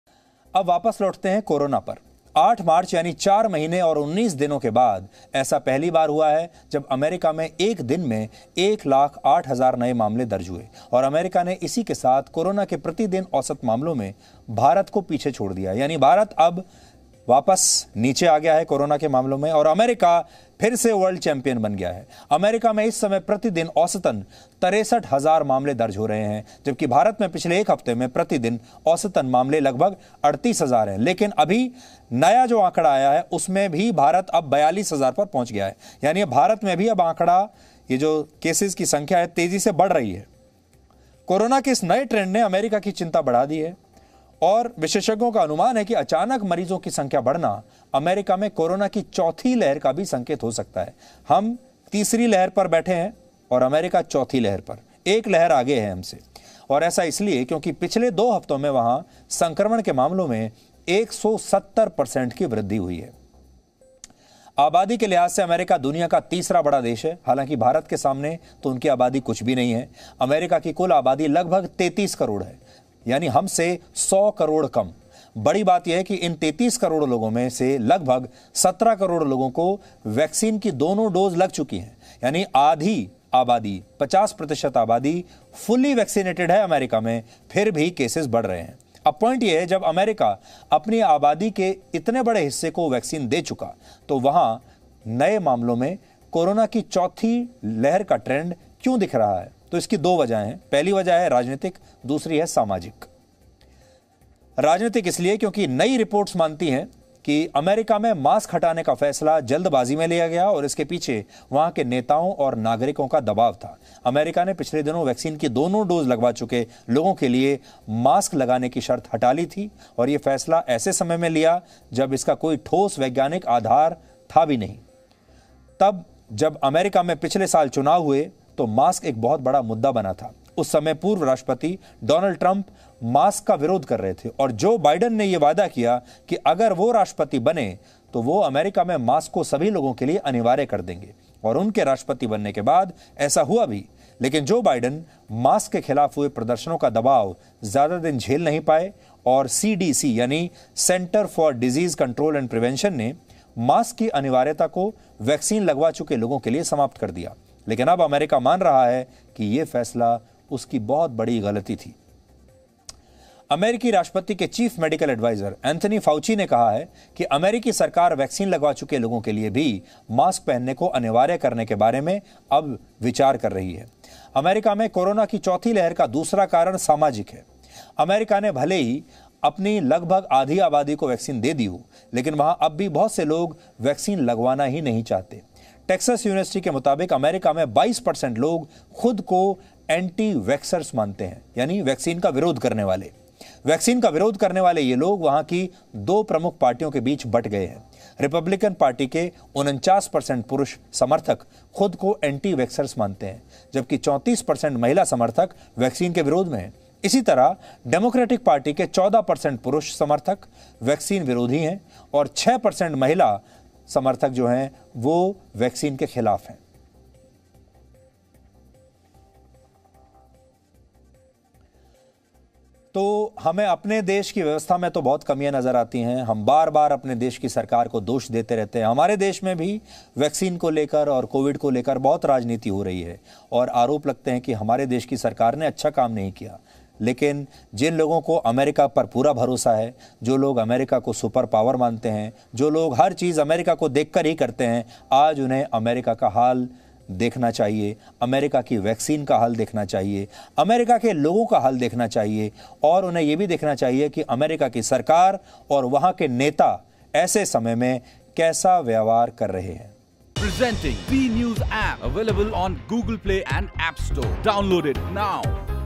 DNA: US मे फिर बेकाबू हुआ Coronavirus, भारत से निकला आगे | Sudhir Chaudhary | COVID-19 | Fourth Wave
DNA: Coronavirus again out of control as America faces fourth wave
Despite the vaccination of 50 percent of the population, cases of corona infection have started increasing rapidly in America. This speed is so fast that it has even left India behind in terms of average per day. This is the fourth wave of corona in America while there is a danger of the third wave in India.

DNA: US मे फिर बेकाबू हुआ कोरोनावायरस
50 फीसदी आबादी का टीकाकरण कराने के बावजूद अमेरिका में कोरोना संक्रमण के मामले तेजी से बढ़ने लगे हैं. ये रफ्तार इतनी तेज है कि इसने प्रति दिन औसत के मामले में भारत को भी पीछे छोड़ दिया है. ये अमेरिका में कोरोना की चौथी लहर है जबकि भारत में तीसरी लहर की आशंका बनी हुई है.